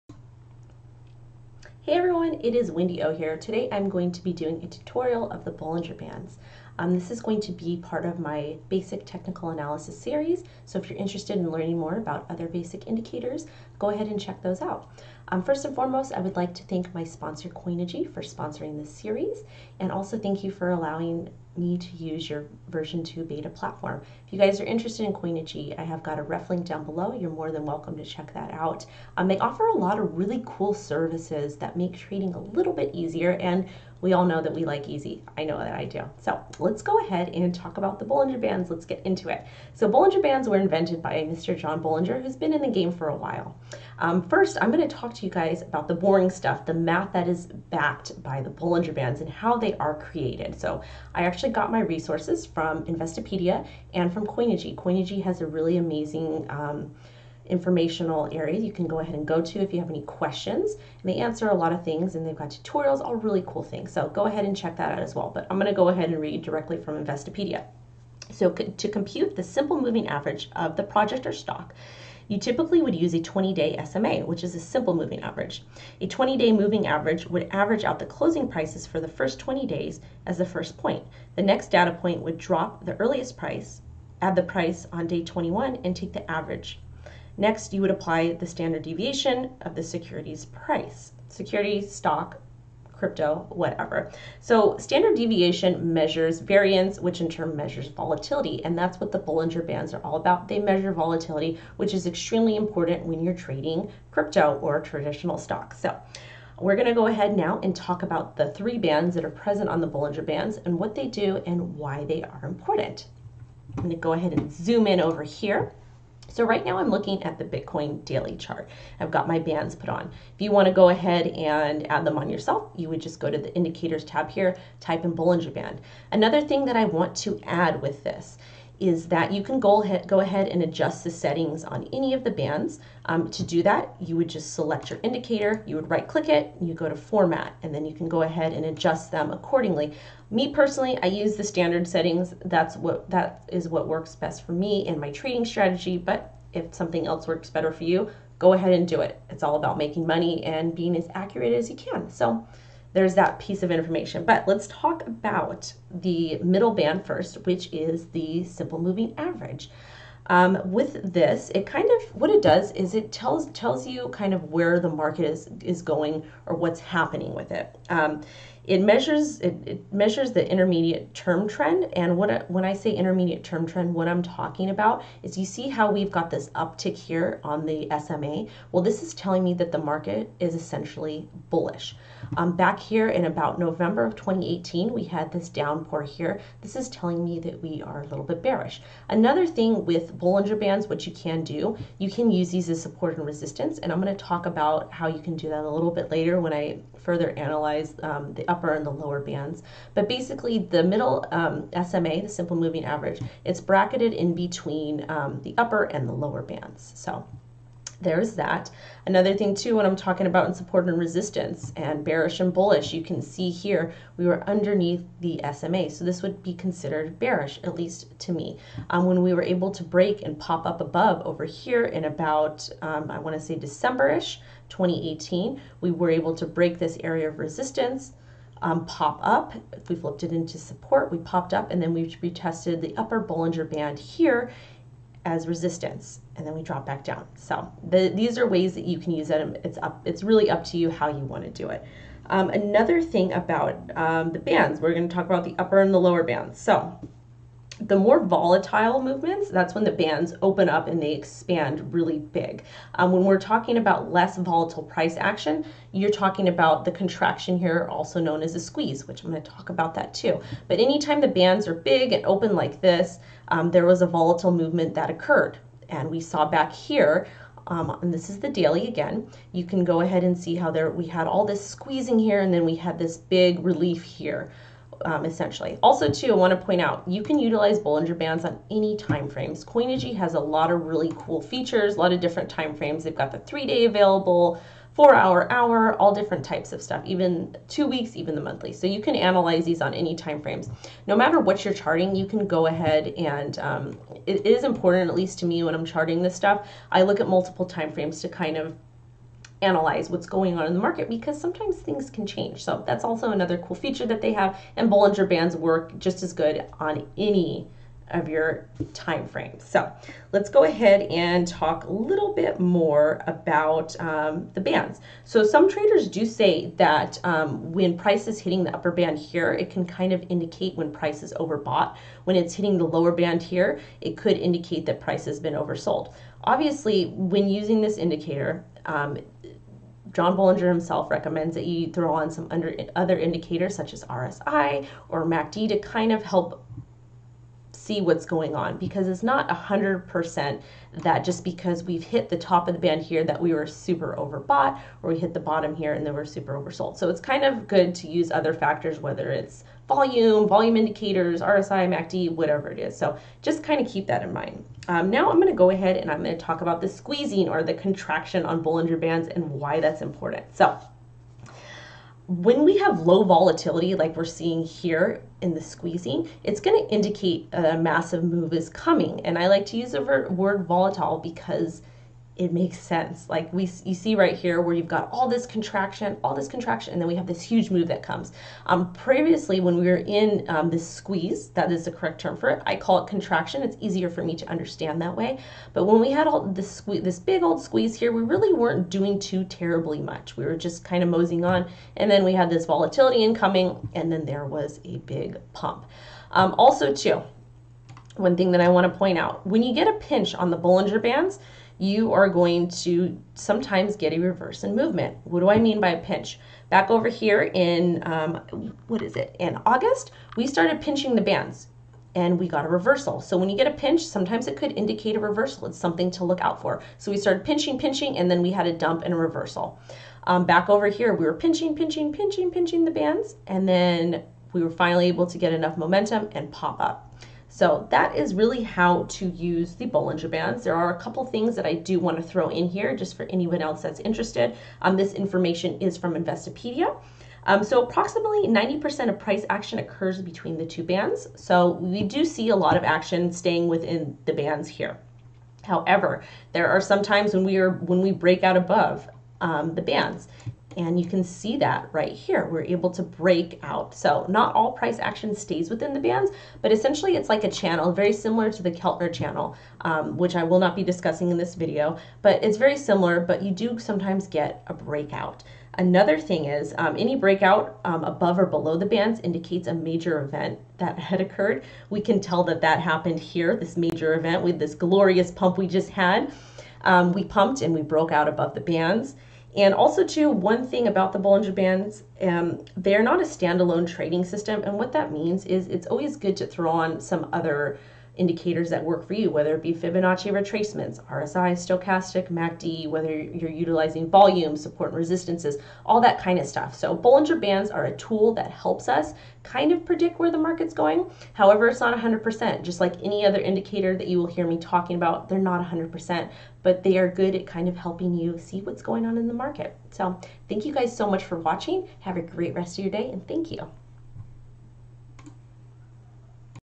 Bollinger Band Tutorial - Bitcoin Technical Analysis
How to use Bollinger Bands on Coinigy V2

Bitcoin Trading Platform. Professional Tools For Cryptocurrency Traders.